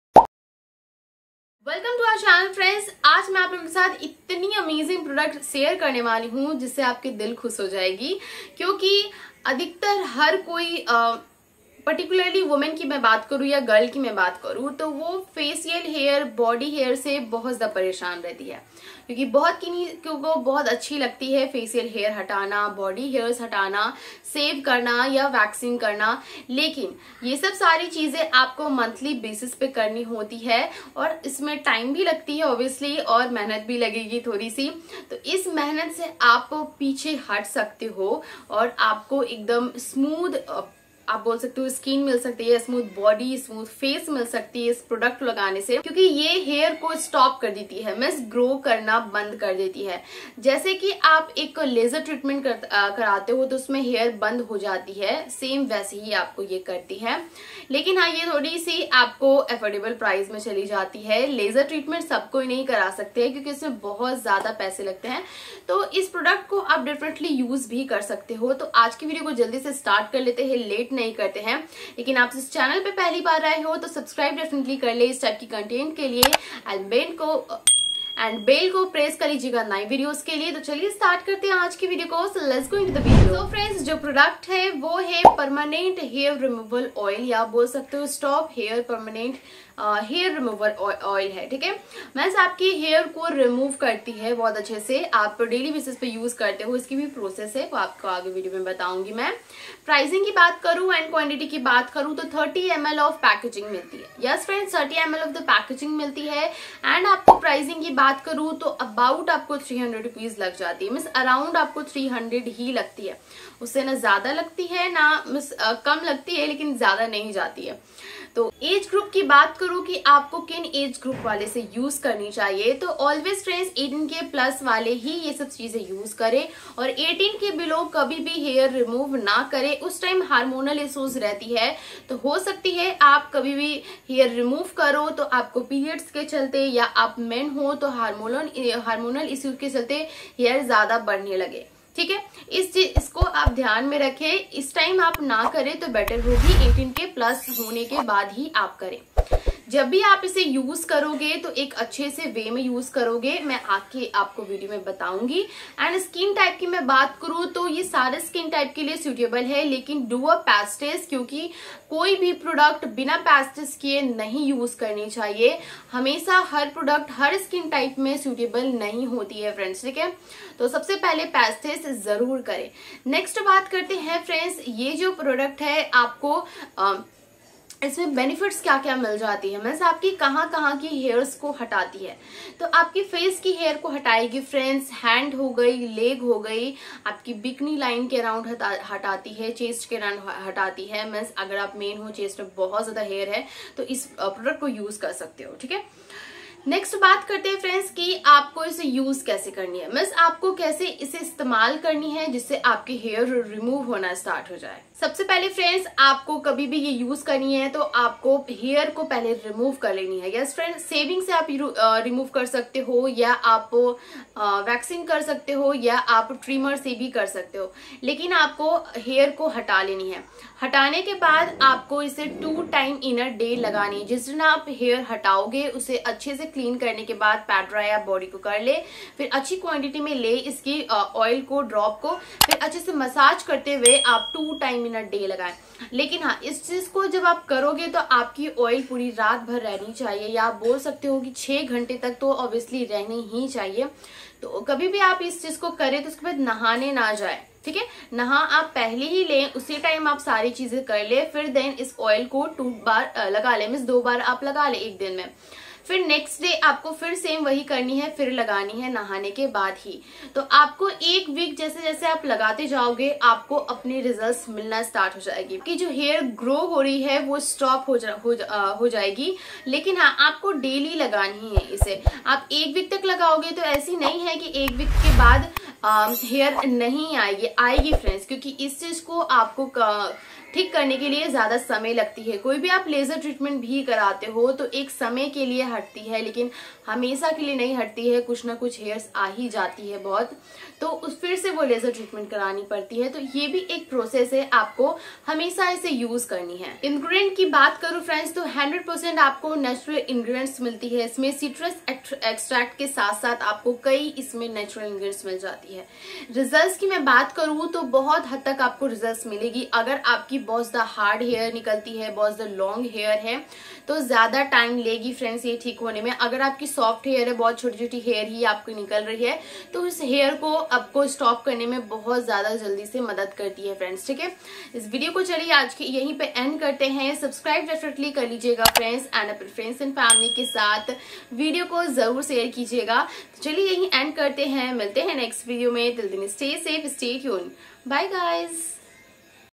don't use glow India permanent hair removal oil , stop hair honest review , glow India stop hair oil
Product Link 👇👇👇
Glow India Stop Hair Removal Oil

Best Cream For Pigmentation (𝙋𝙪𝙧𝙘𝙝𝙖𝙨𝙚 𝙇𝙞𝙣𝙠)

Best Cream For Skin Lightening(𝙋𝙪𝙧𝙘𝙝𝙖𝙨𝙚 𝙇𝙞𝙣𝙠)

welcome to our channel friends I am shalini and I will tell you all about this product thats helps to cure your problem easily and effective and Today's product is glow India Stop Hair Removal Oil